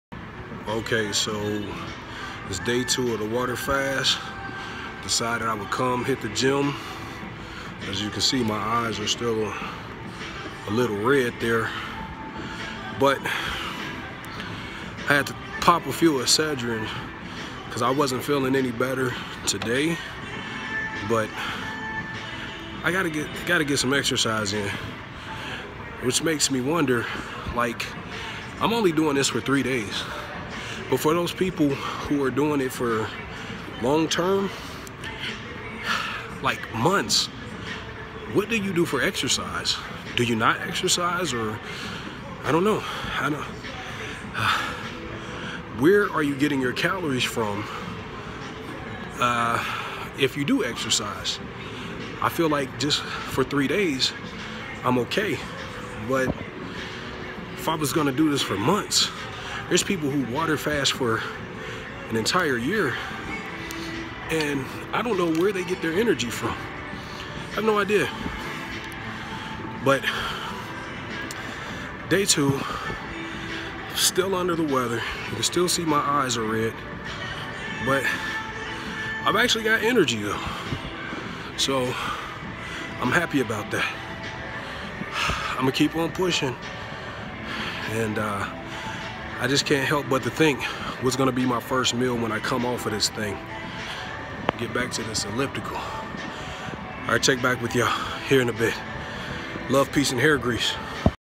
Day 2 water fasting working out while sick
It’s say 2 for my water fast and overall I’m not very hungry. I’m still feeling a little under The Get Down weather but I had to get some exercise. Wondering how people that fast long term get exercise? Let me know!